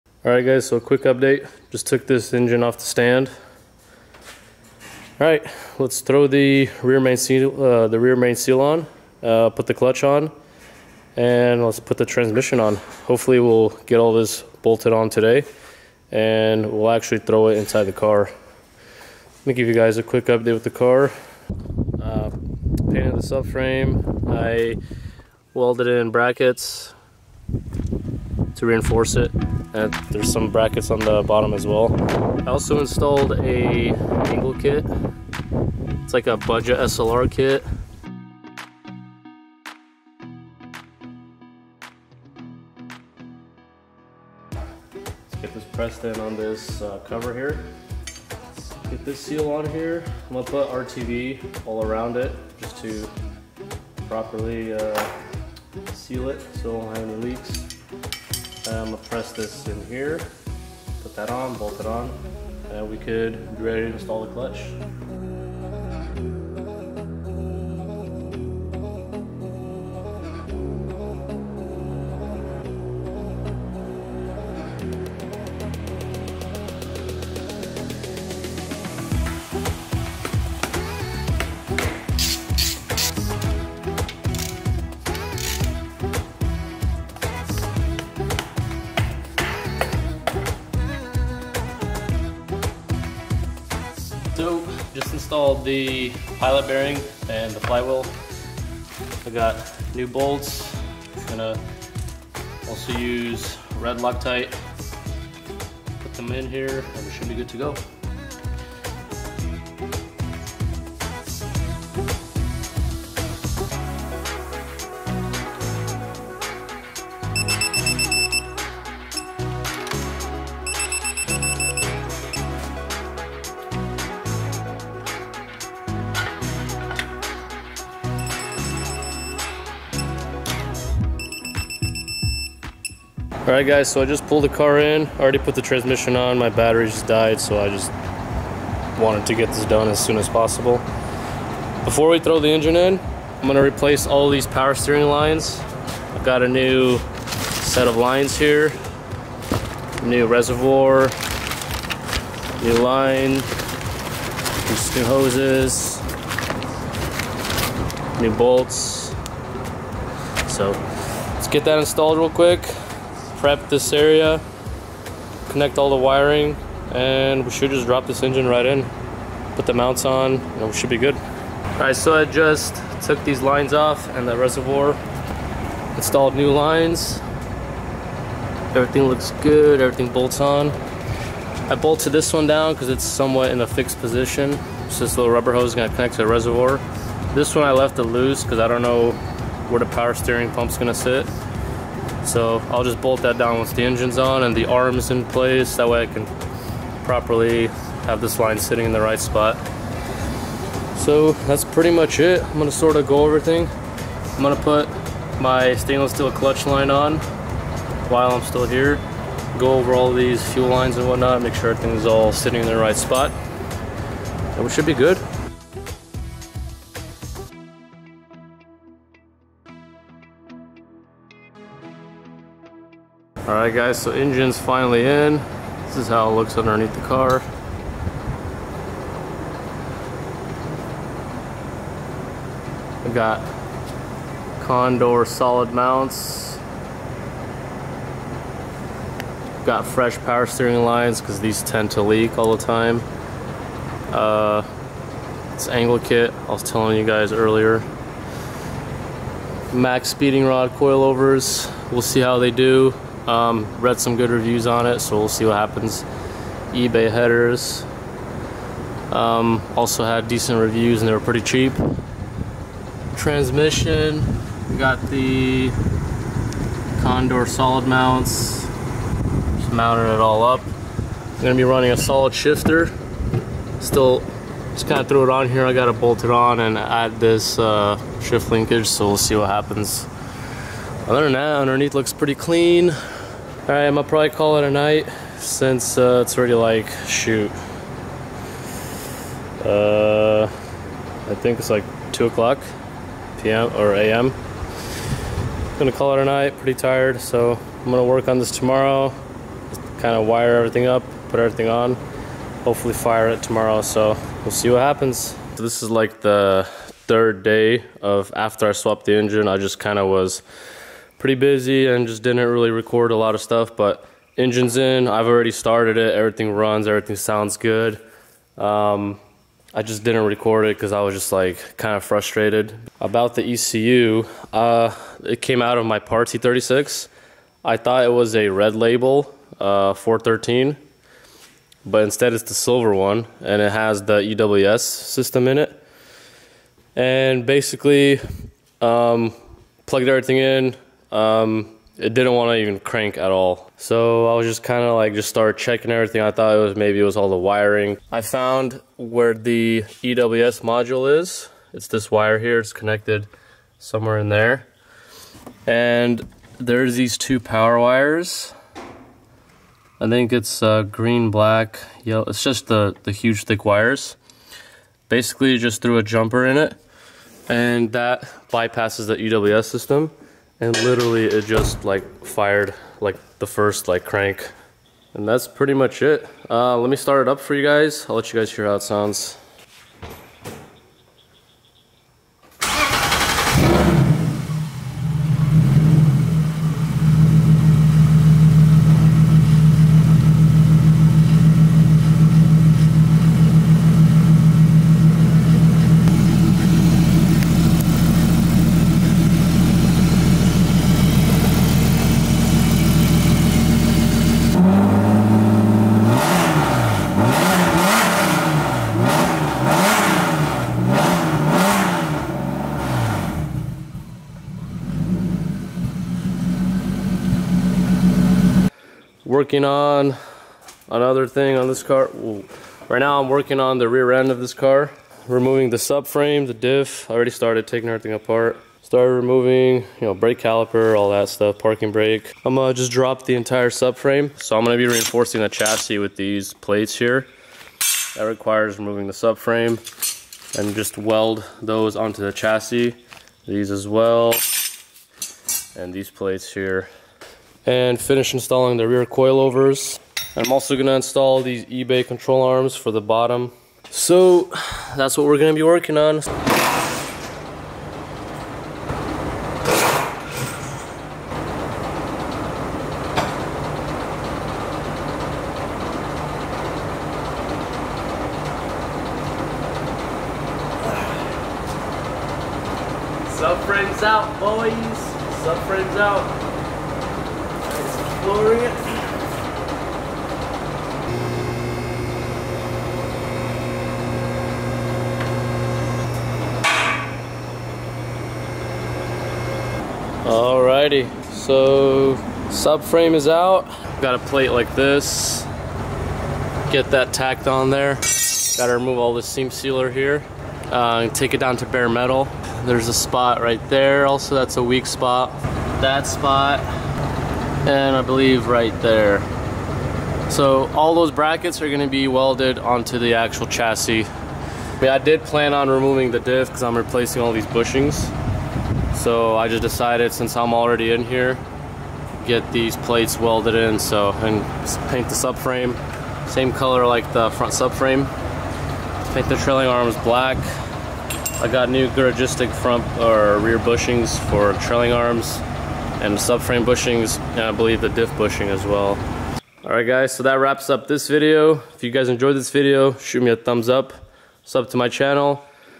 E36 DRIFT BUILD | M52B30 INSTALL + FIRST START | PART 4
Long overdue video! At this point the e36 is running strong and ive already been to my first event on it ( footage will be coming soon  ). In this video ill be showing you how I Installed the clutch and fully dressed the motor and got it to run. Ran into a few issues but now everything is now resolved!

LIKE/COMMENT/SUBSCIRBE/SHARE